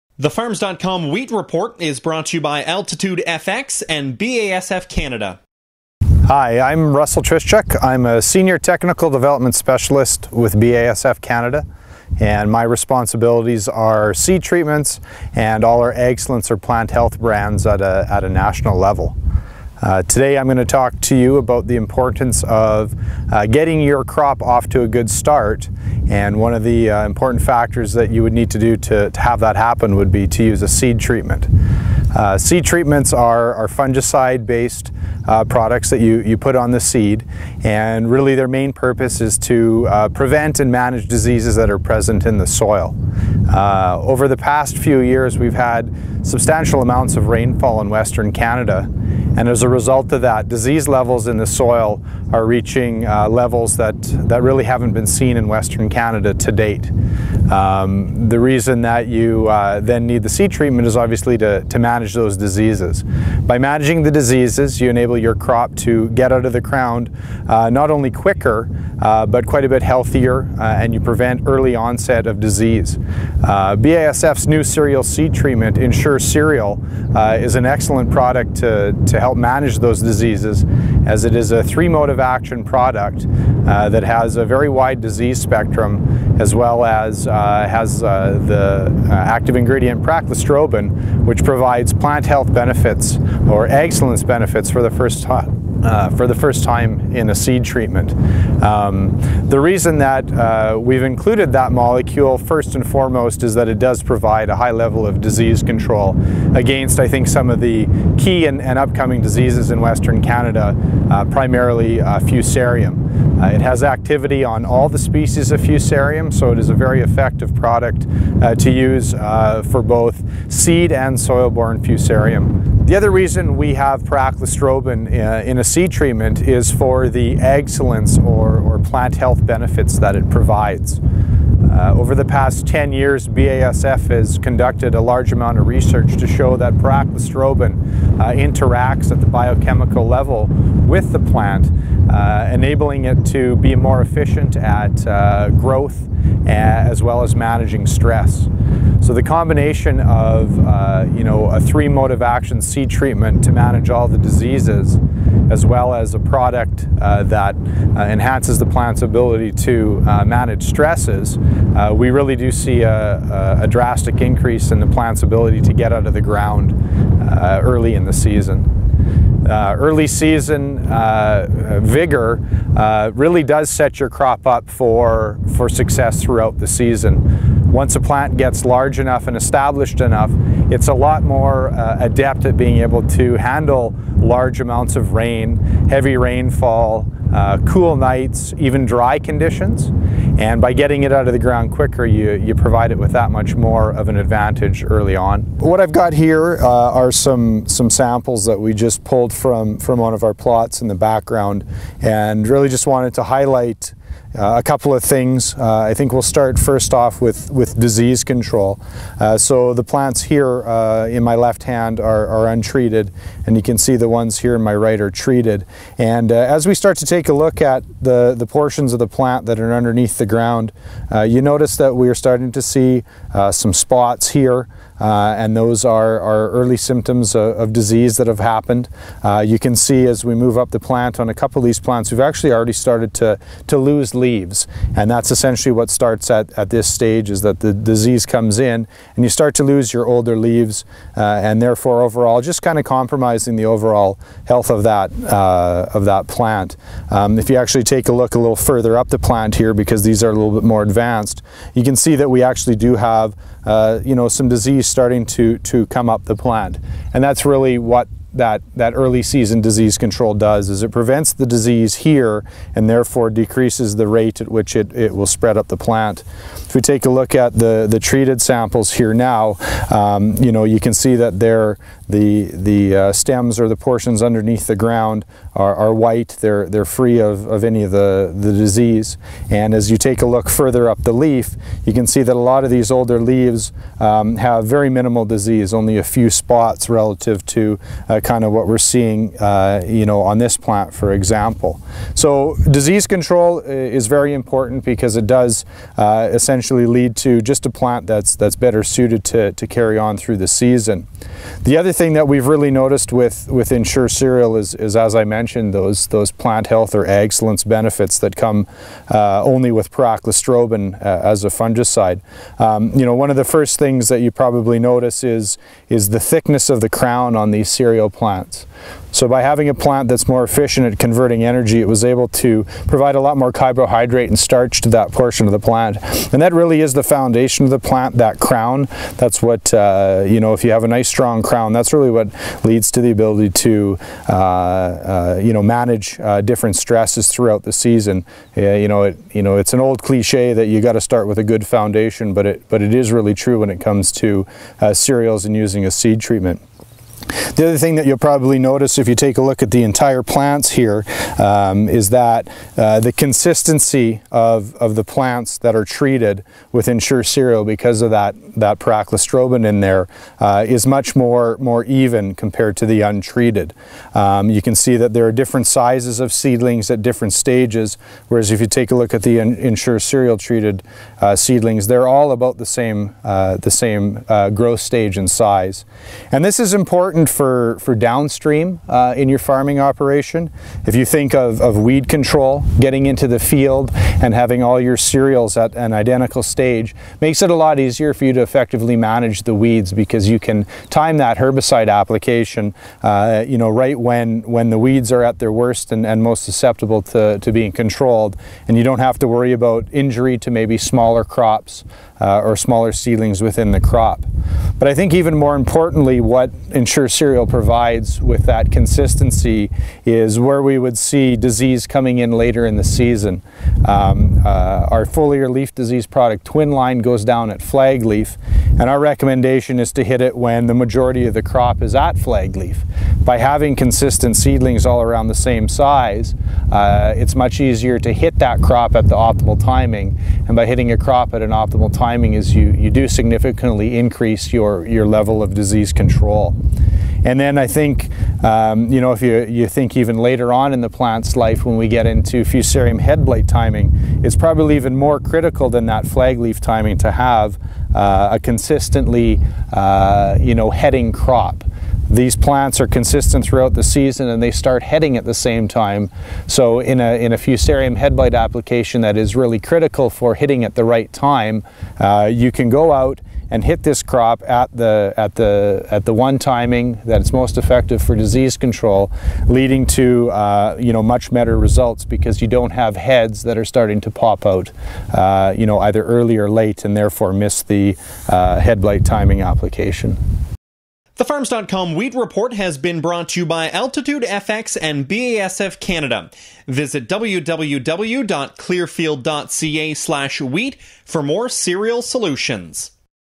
Farms.com Wheat Report: The Importance of Seed Treatment For Optimum Plant Health.
Russell Trischuk with BASF Canada explains how producers can get their wheat crop off to the best growing start with seed treatment.